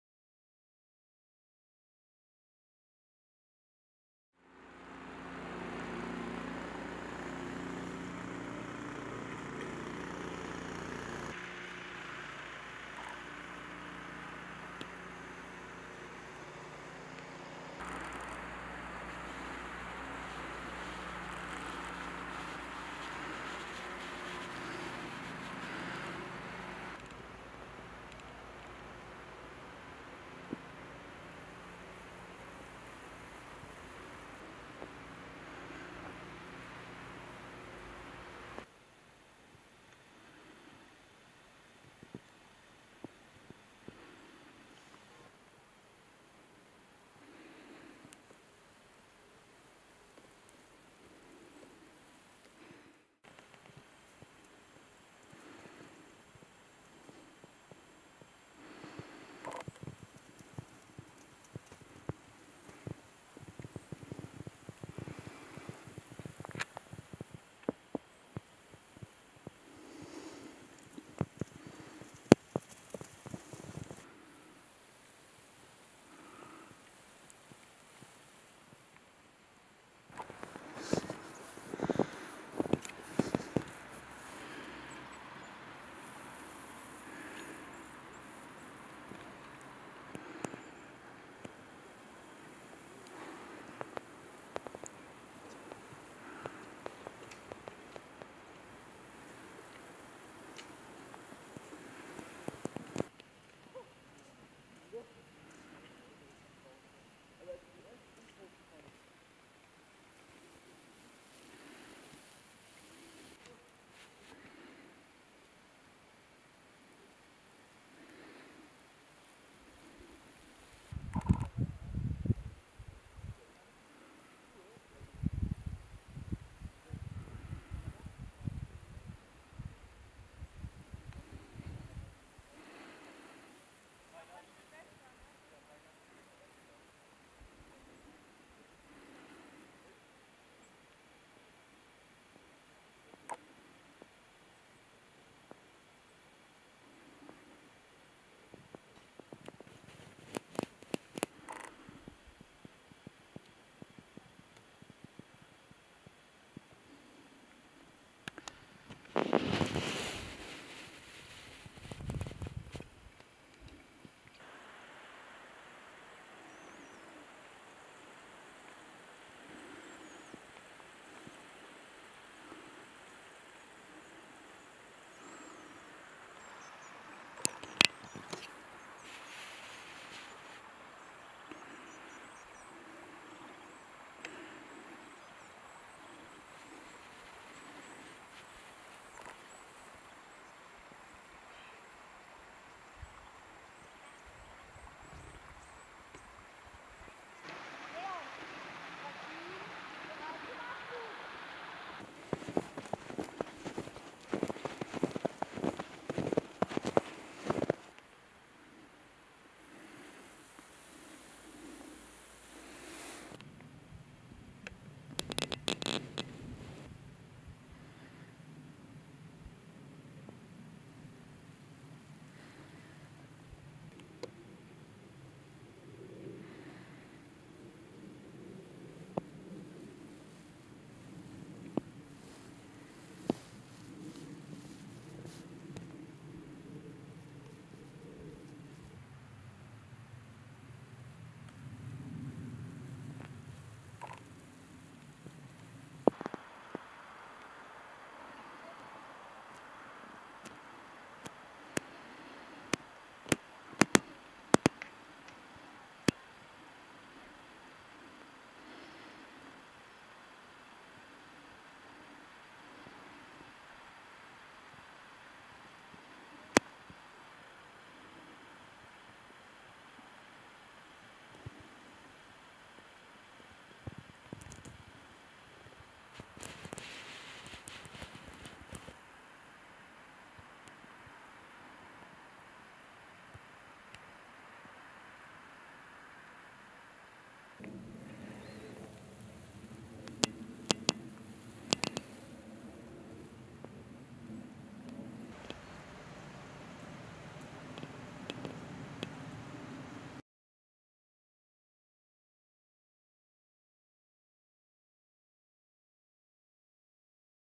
Winterwanderung von Altenau nach Clausthal über die ehemaligen Bahngleise
Eine leichte Wanderung, die man in Altenau machen kann, ist entlang der Bahngleise zu gehen (oder auch mit dem Fahrrad oder im Winter mit Langläufern).
Eine schöne, leichte ebenerdige Wanderung. Bis nach Clausthal sind es ca. 7km, das heißt, wenn man sie ganz laufen möchte,m muss man etwas Zeit mitbringen.